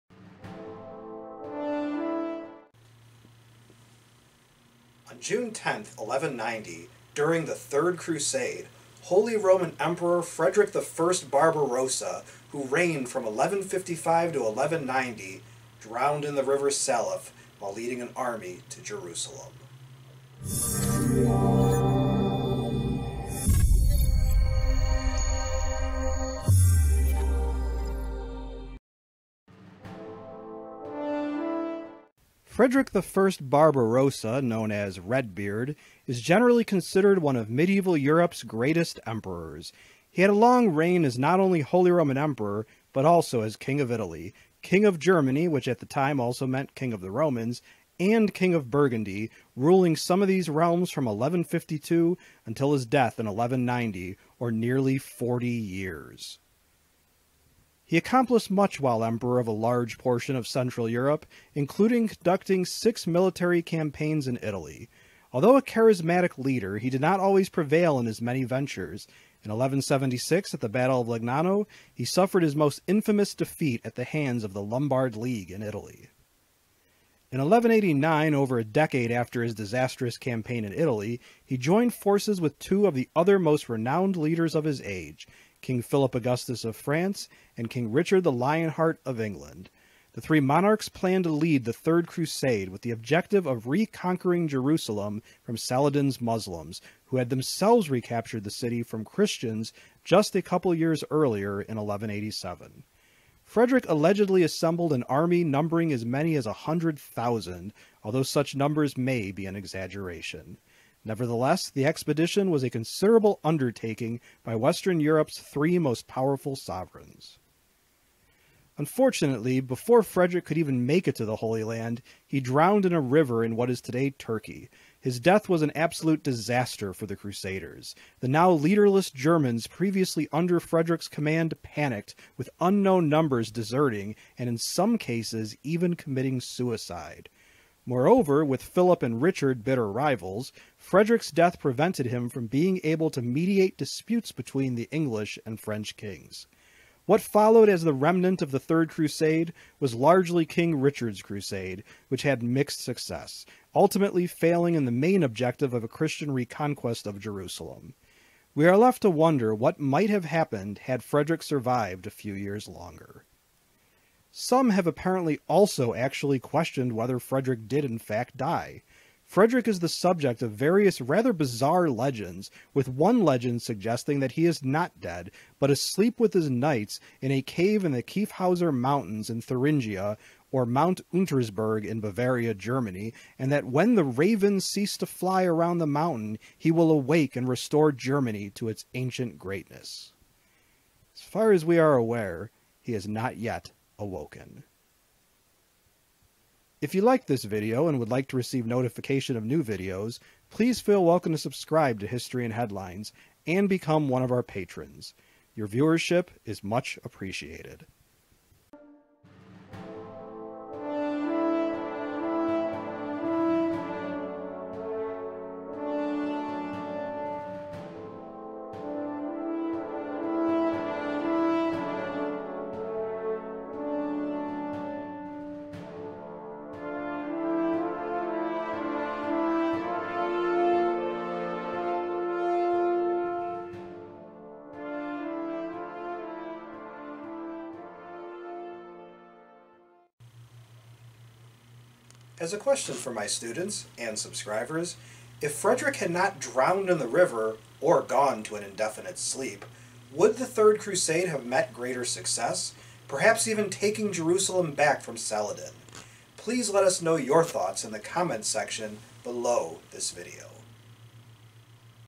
Emperor Frederick I Barbarossa drowns during Third Crusade!
On June 10, 1190, during the Third Crusade, Holy Roman Emperor Frederick I Barbarossa (r. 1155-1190) drowned in the river Saleph while leading an army to Jerusalem.

The text narrated in this video is from the original article

For sourcing for that article, please refer to the “Historical Evidence” section of the article on our main site on which this video is based.

Author: Dr. Zar

Video edited using Movie Maker.

Video footage recorded by Dr. Zar on a Sony Handycam HDR-CX440.

Image sources:

The following images are in the public domain for various reasons.